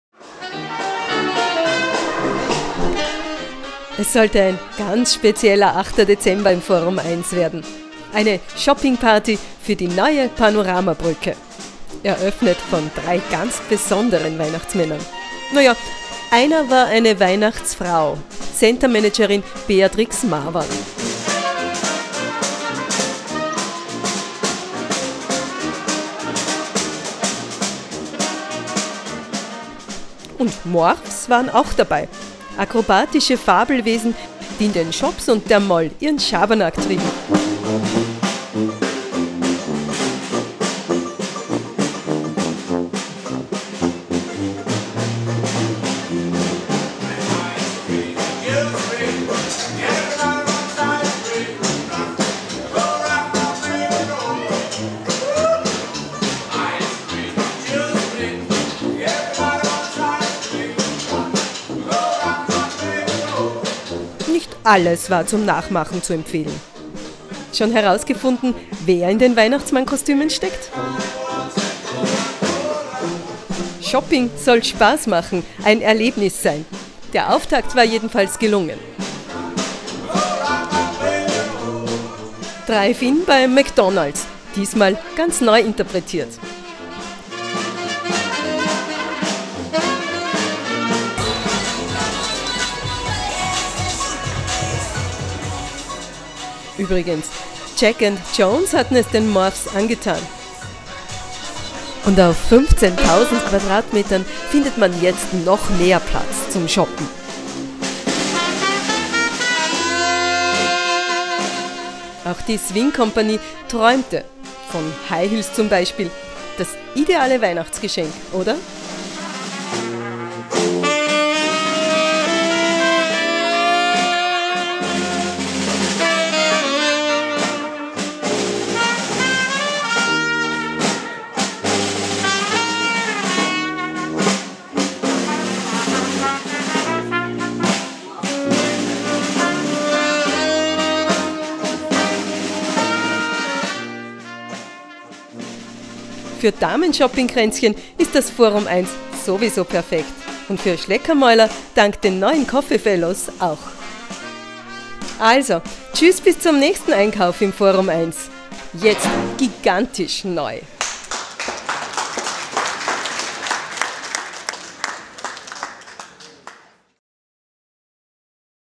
FORUM 1 Panoramabrücke Eröffnung.mp4
Sie misst 22 Meter in der Länge, acht Meter in der Breite und wiegt 70 Tonnen: Eine Panoramabrücke, die an die elegante, gläserne Fassade des FORUM 1 andockt und beide Gebäudeteile miteinander verbindet, ist neues Wahrzeichen am Salzburger Hauptbahnhof. Offiziell eröffnet wurde die neue imposante Passage des FORUM 1 am Donnerstag, 8. Dezember mit Weihnachtsmännern, Morphs und der Swing Company.